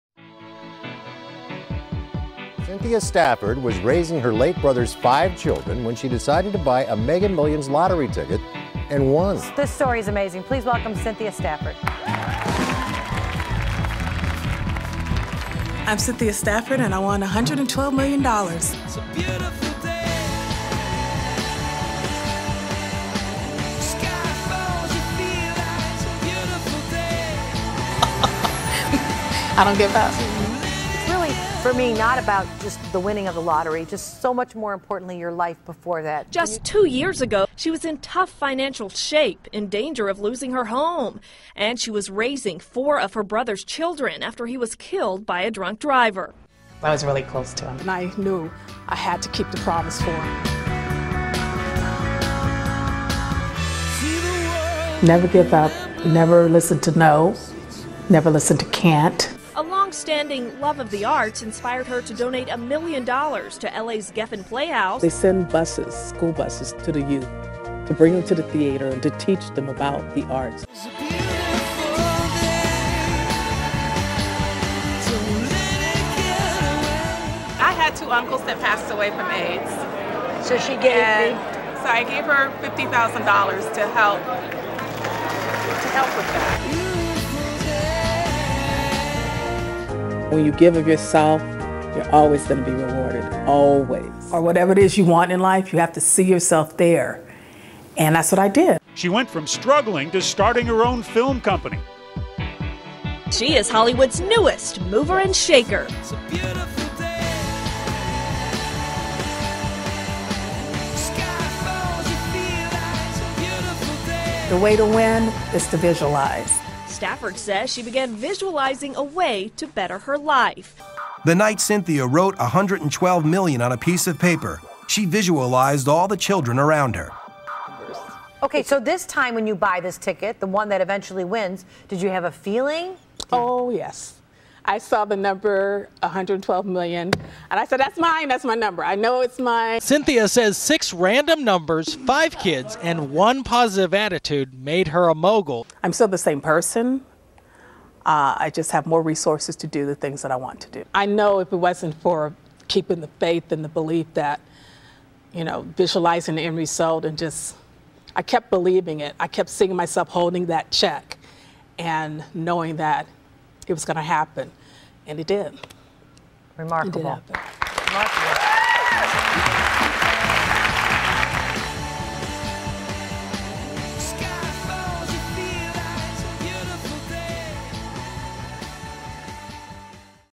Cynthia Stafford: Never Listen to No
Cynthia Stafford's story, from caring for her brother's five children to winning $112 million in the California Lottery to her own movie production company to a charitable giver and patron of the arts.